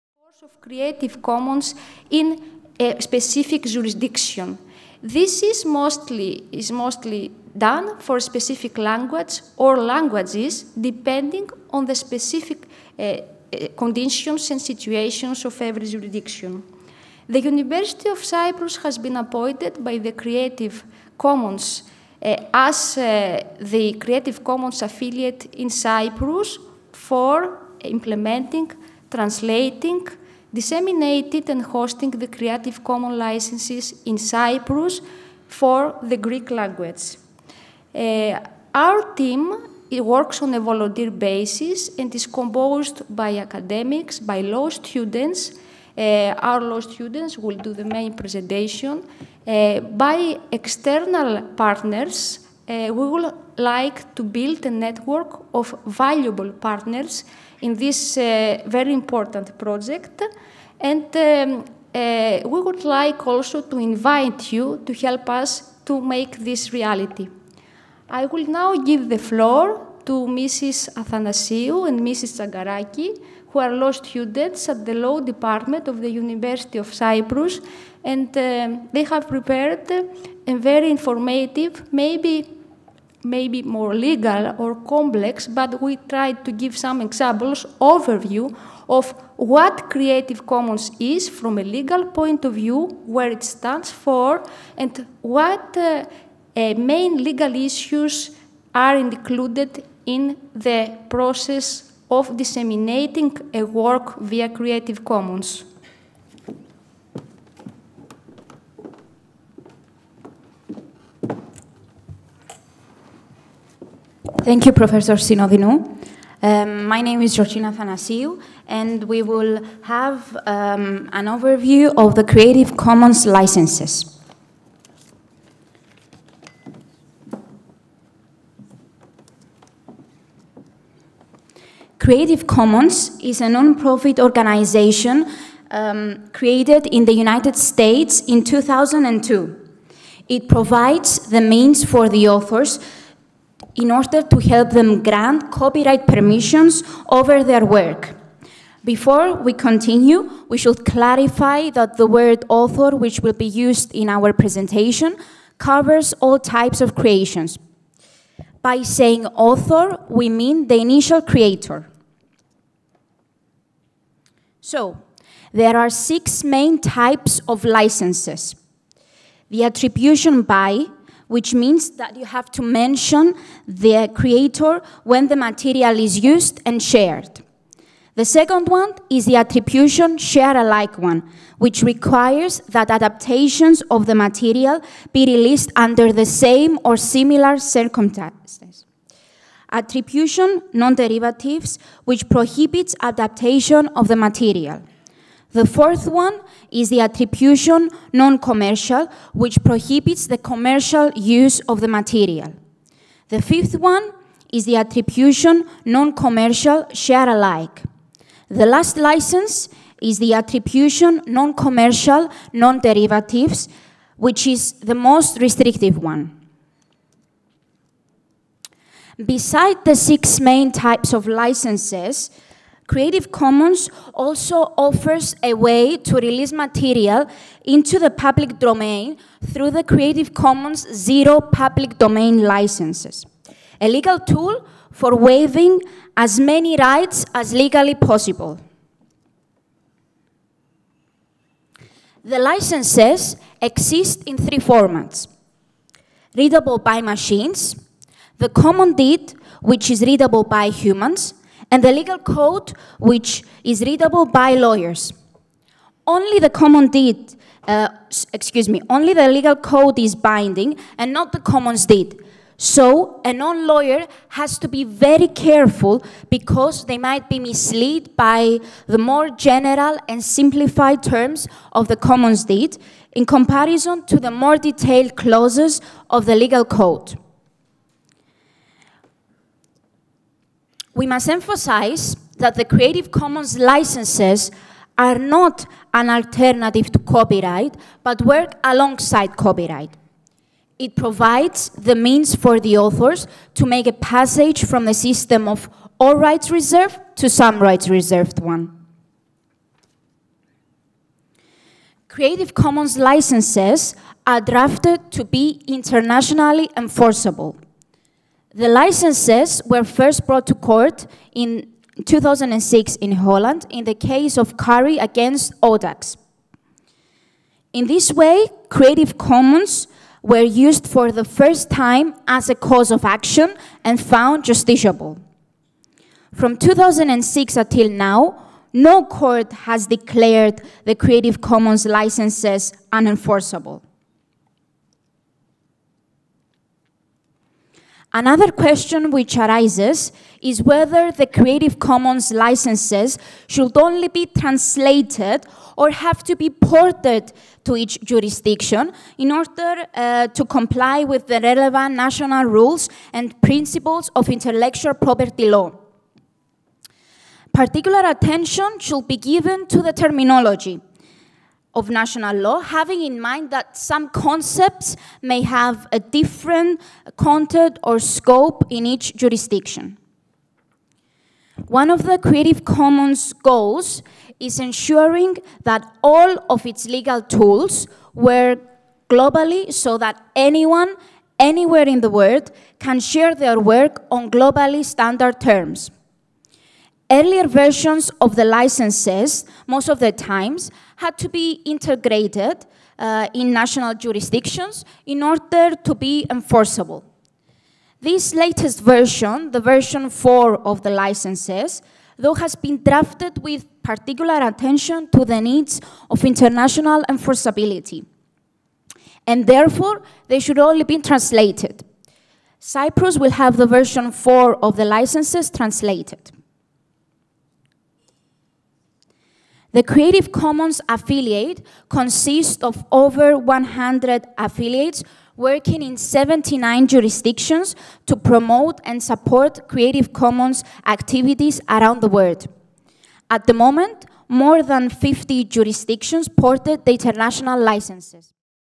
Creative commons: a legal overview
National workshop for Open Access in Cyprus 23/10/2015
By T. Synodinou, I. Tzagaraki,  G. Athanassiou  and CC Cyprus working group